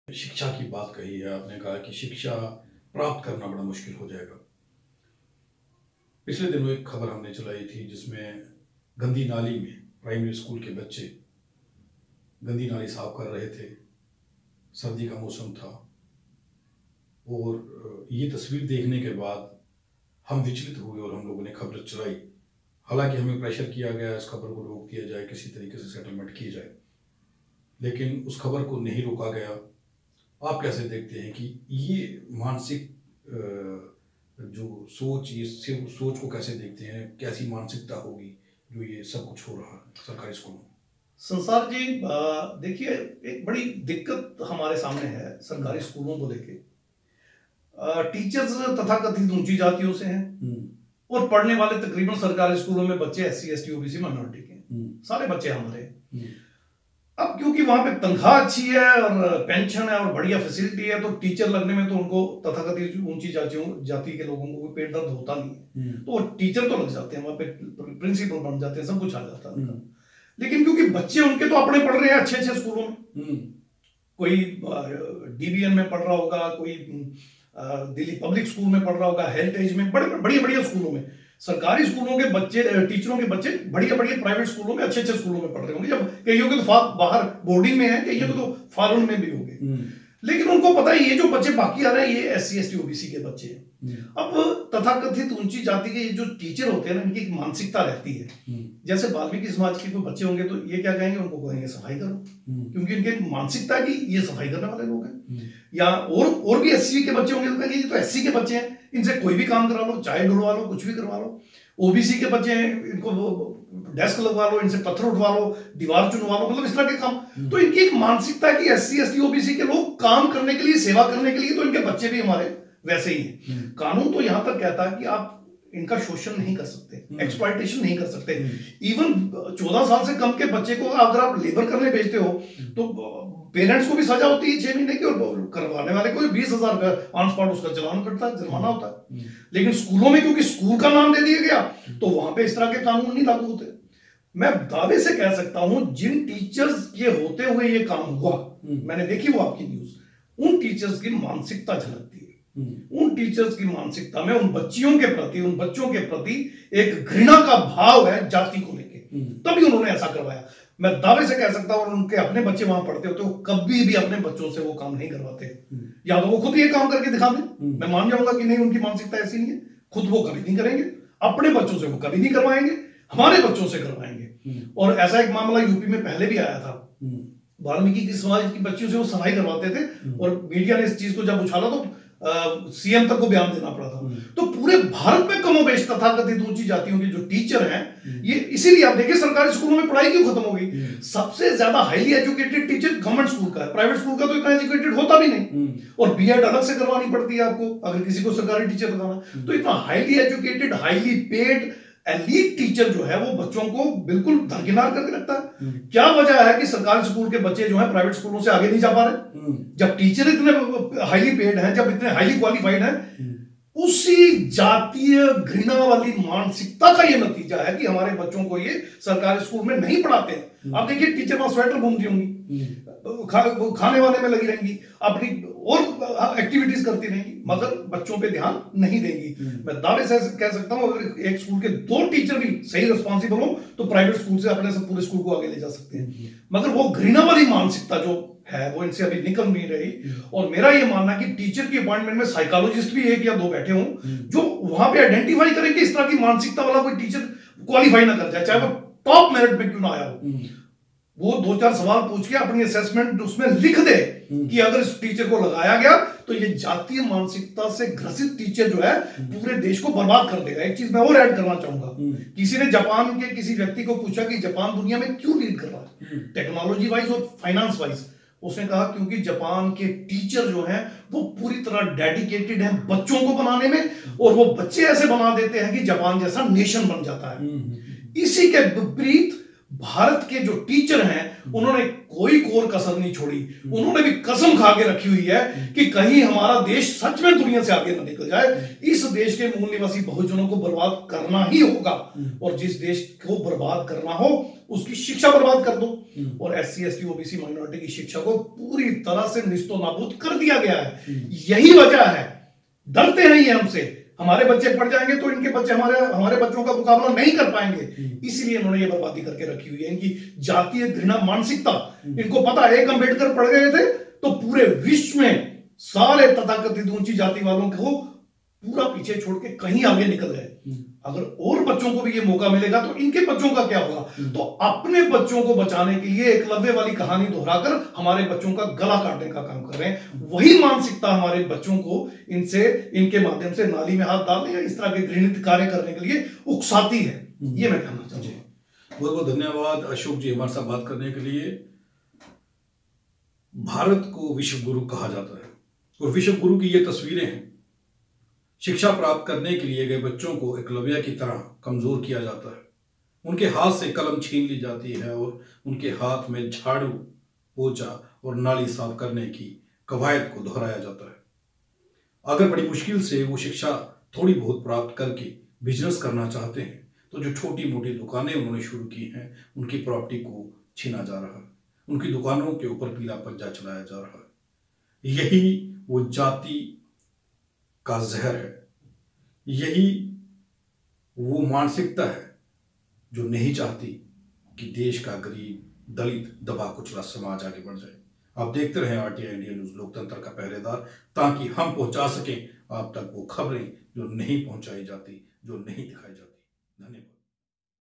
गवर्नमेंट प्राइमरी स्कूल मक्खनपुर में बच्चों से नाली साफ करवाने वाली खबर को देखने के बाद
Ashok Kumar Basotra